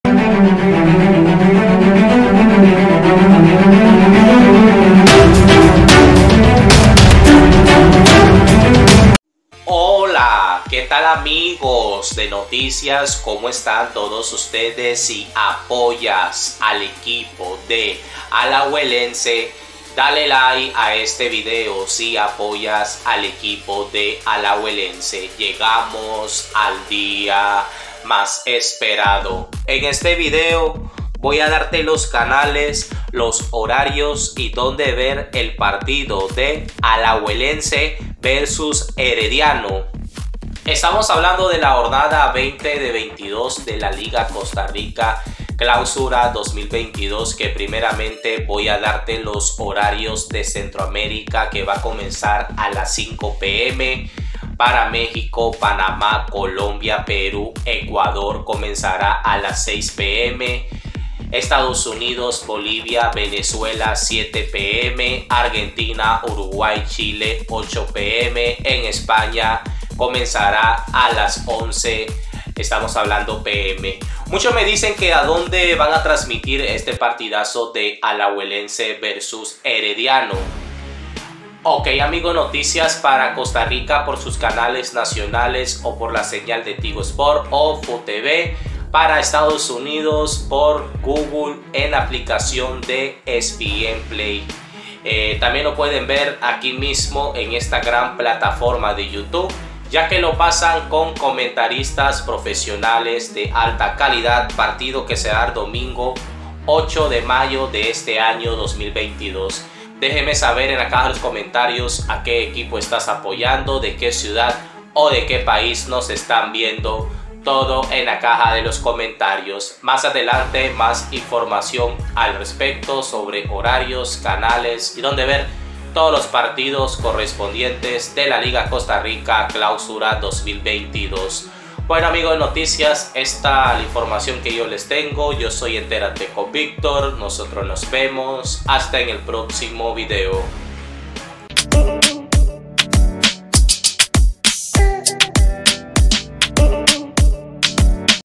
Donde ver Alajuelense vs. Herediano en vivo, por la Jornada 20, Liga Costa Rica 2022
Ver La Liga vs. Herediano, Donde ver, en que canal ver, hora, día, en vivo, Herediano vs. Alajuela Domingo 8 de Mayo 2022, les dejo los canales con país y donde ver hoy el partido Liga Deportiva Alajuelense vs. Heredia Jornada 20 de 22 Liga Costa Rica, Clausura 2022, desde el estadio Alejandro Morera Soto.